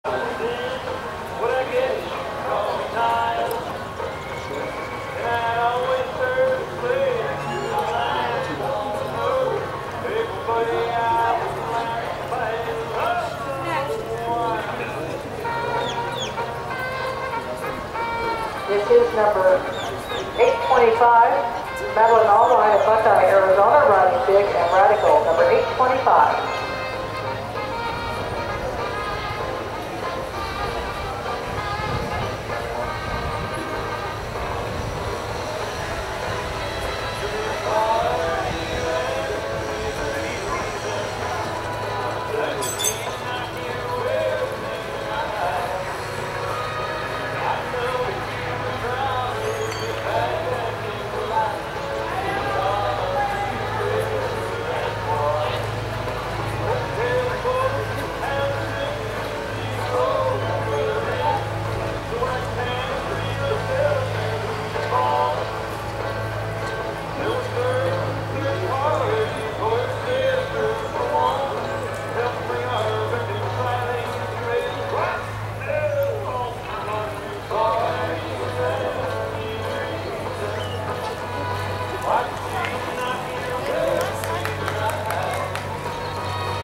Madeline Alewine Horsemanship
Horsemanship with Big And Radical at the 2015 Sun Country Circuit. This is Alvin's first Horsemanship class and first Horsemanship pattern. In this class of 23 we placed 5th in the Novice Youth Horsemanship.